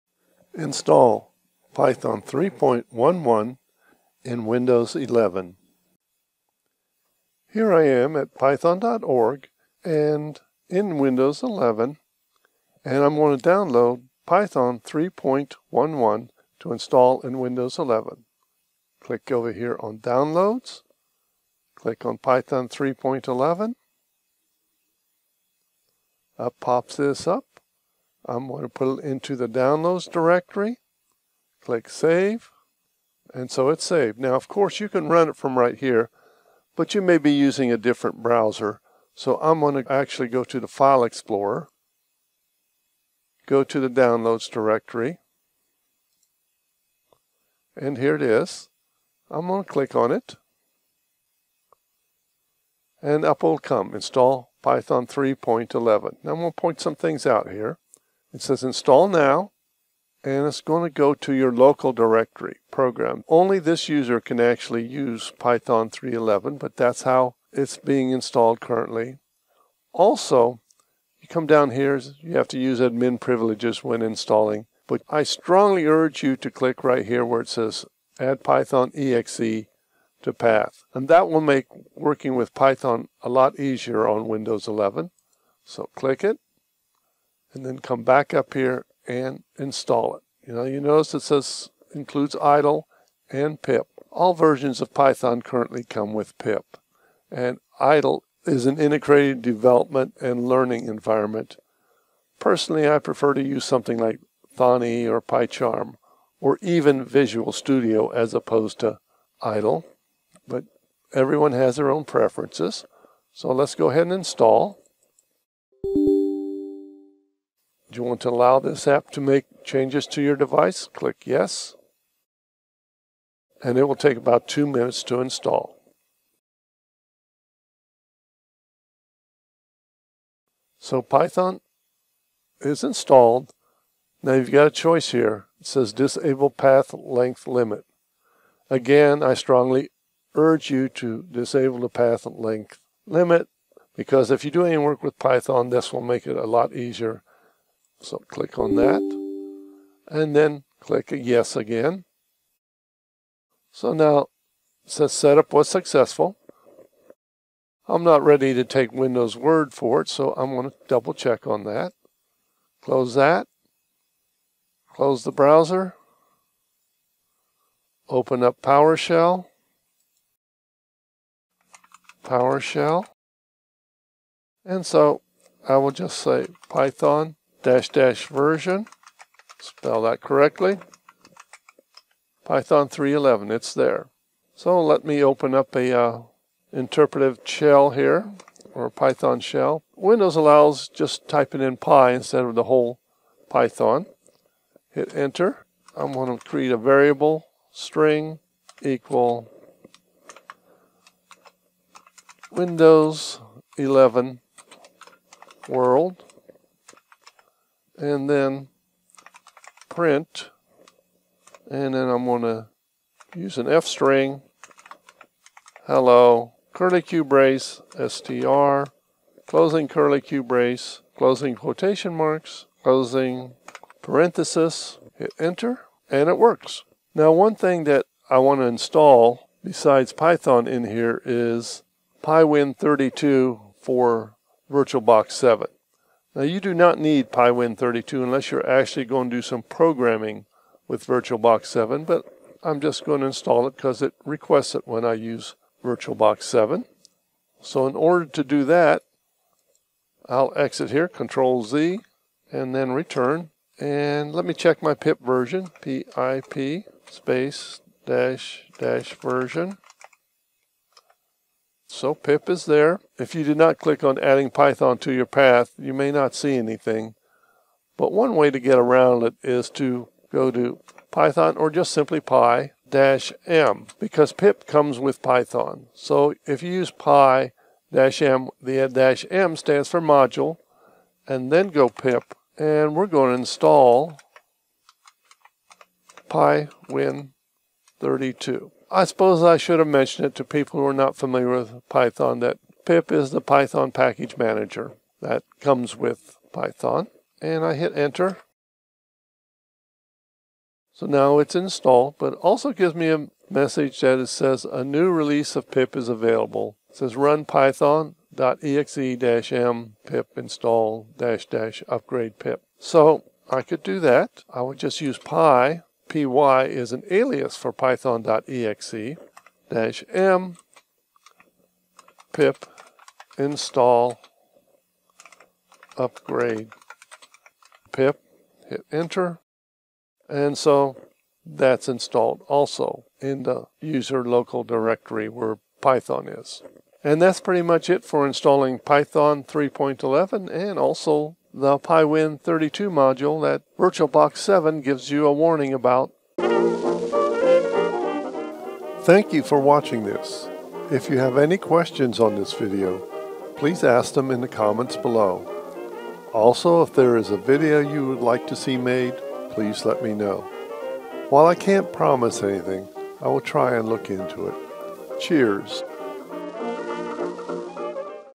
Install Python 3.11 in Windows 11; access pip module, install pywin32 for VirtualBox 7
Ever wanted to learn Python but didn't know how to install it in Windows. Or did you get the Warning Message in VirtualBox 7 about missing a core dependency- pywin32 and didn't know how to install it. Or wanted to use the pip (Python packaging manager) module that comes downloaded and installed with Python 3.11.1.  Well this video demonstrates how to solve all 3 of these issues in a little over 6 minutes. Also it demonstrates a small 'Hello World' program to verify everything has gone well with the install.

0:00 Download Python & Install Python
2:46 Verify Python works- Hello World
4:13 Install pywin32 for VirtualBox 7